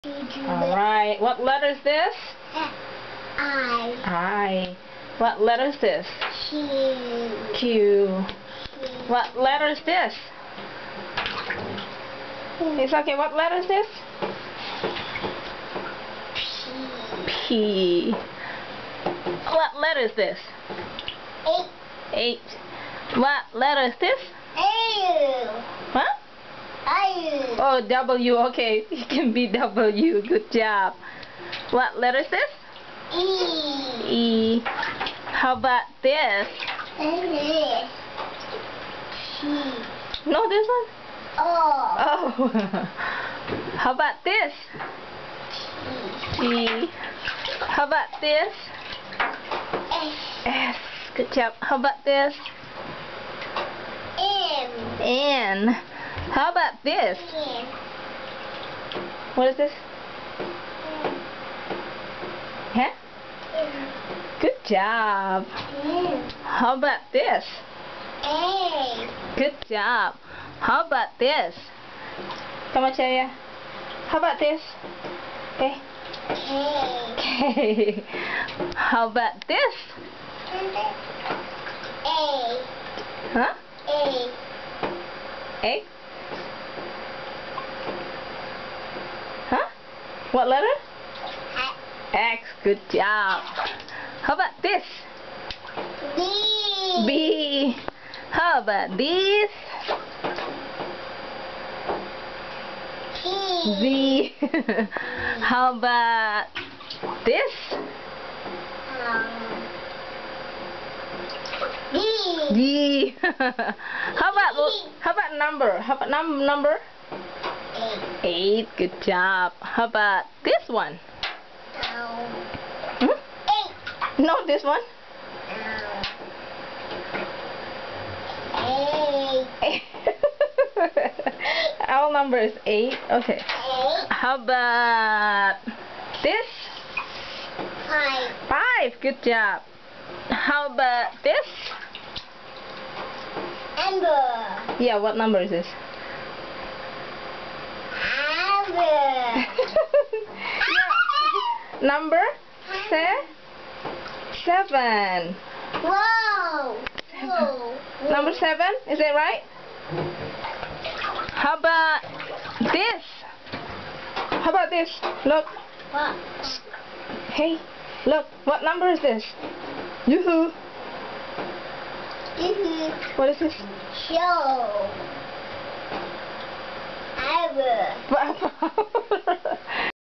I know my letters and numbers
Kamajaya, 27months, knows his letters and numbers pretty well. He sometimes mistakes 0 for "o" or  "W" for "M" or the other way around. He has a little class in front of computer with his poppa. And before I knew, he brought me my Tintin book one day and said,"momma, T-I-N..T I N T I N".